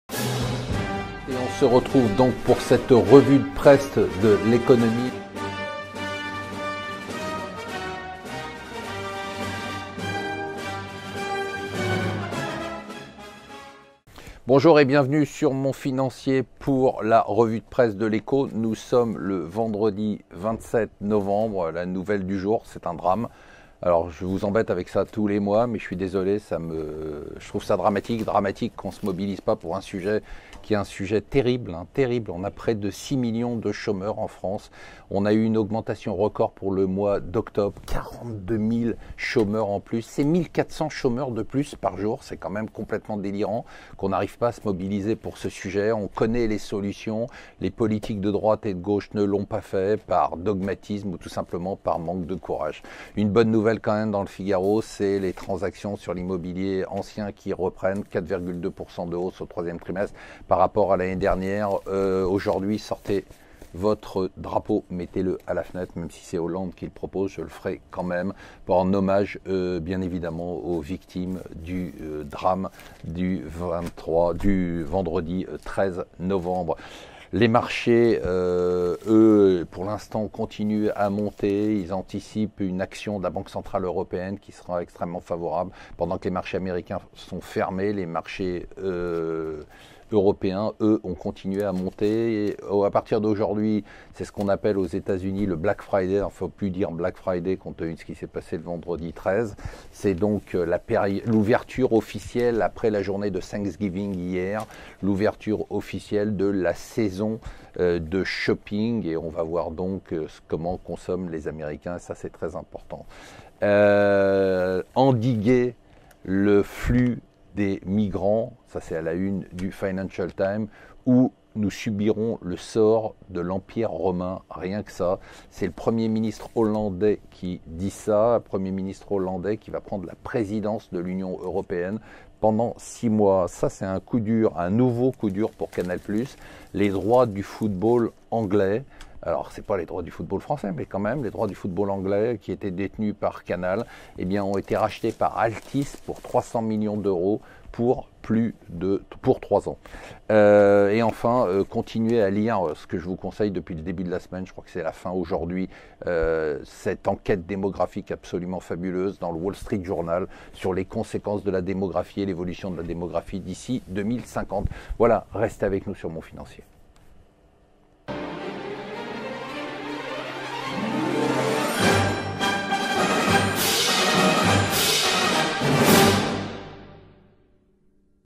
Dramatique
Le drame du chômage, L'ISF dans la campagne, Drahi contre Bolloré, Le carnaval COP21